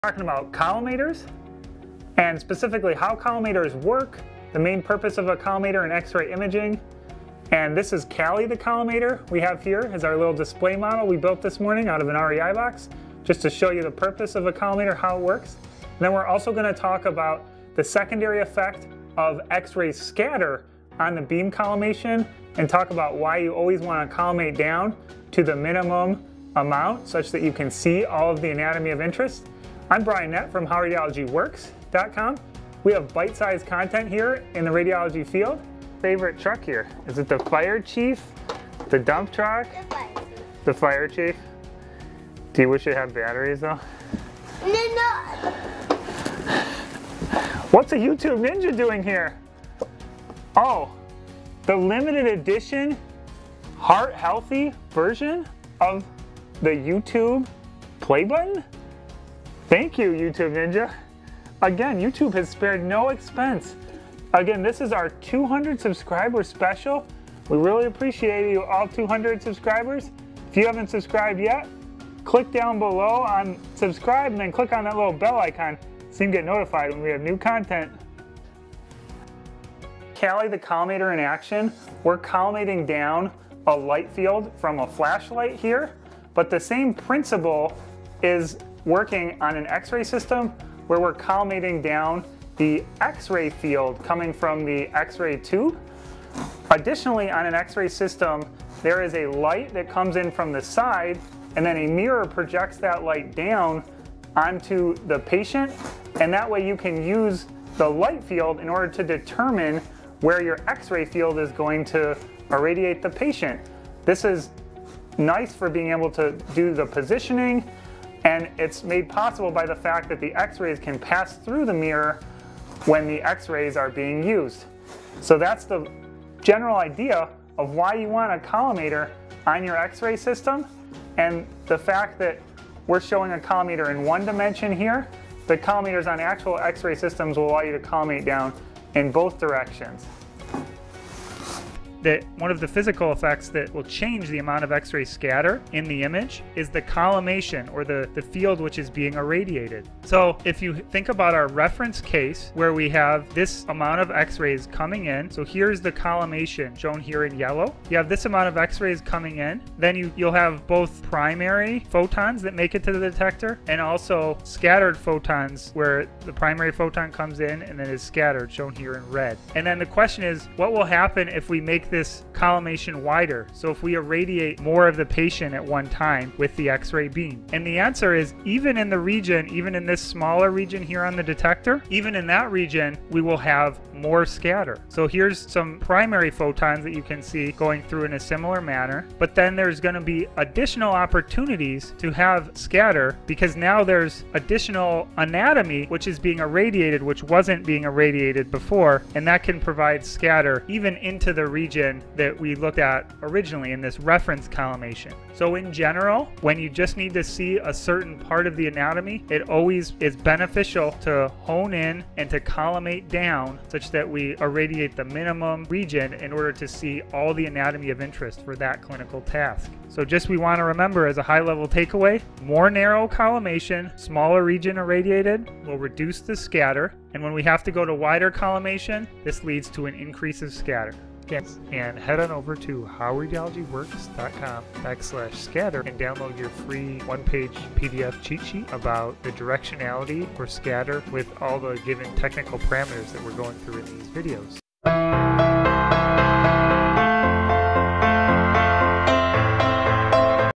X-ray Scatter FOV (collimators impact on scatter) [200 Subscriber Special for Rad Techs]
X-ray scatter is dependent on the FOV (Field of View). The FOV is controlled by the collimator in x-ray radiography systems. The collimator typically contains lead plates which restrict the x-rays coming out of the x-ray tube. This is controlled by the Radiologic Technologist (Rad Tech) or Radiographer.

In this video we introduce the basic idea of collimating a beam and then describe the secondary affect the beam collimation has on x-ray scatter on the measured x-ray image.

Chapters:
00:00  Introduction Collimators
00:38  200 Subscriber Special
01:19. Collimator in action (Demo with flashlight)
02:20  Scatter dependence on collimation
03:52. PDF cheatsheet download

Depending on the required imaging area for a given clinical task we can change the coverage of x-rays irradiating the body by opening or closing collimator blades. This is termed the x-ray field and is typically also visualized with a light field covering the same area on the patient.

As shown on the picture, a narrow collimation (ie. a small x-ray field) results in smaller yellow area and a smaller volume of the patient where scatter can be generated.

If we increase the size of the x-ray field, (i.e. make the yellow irradiated regions larger) then more of the body is irradiated at once.

Since more of the tissue in the body is irradiated, and there is some probability of Compton scatter for all of this tissue, there will be more x-ray scatter measured on the detector.

In general, when we just need to see a certain part of the anatomy, it is always beneficial to collimate down, so that we are at the minimum region needed to see all the anatomy of interest for that clinical test.

Narrower collimation to a smaller region irradiated will reduce scatter, and when we have to go to wider collimation, this will lead to an increase in X-ray scatter.